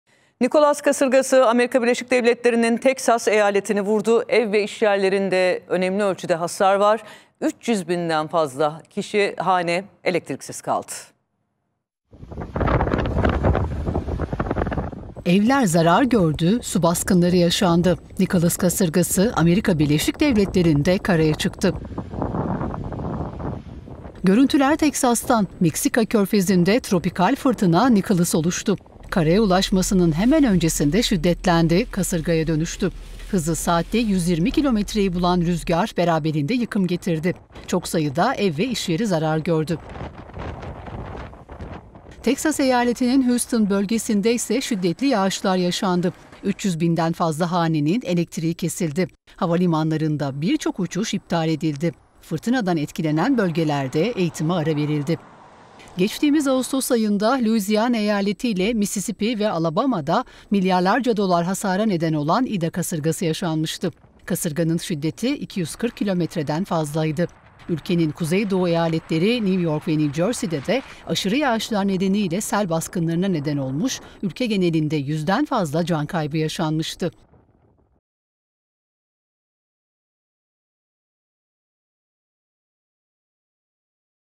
Yine ABD Yine Felaket! Teksas Karıştı
ABD'nin Teksas eyaletini Nicholas kasırgası vurdu. Teksas'ta birçok ev ve işyeri zarar gördü.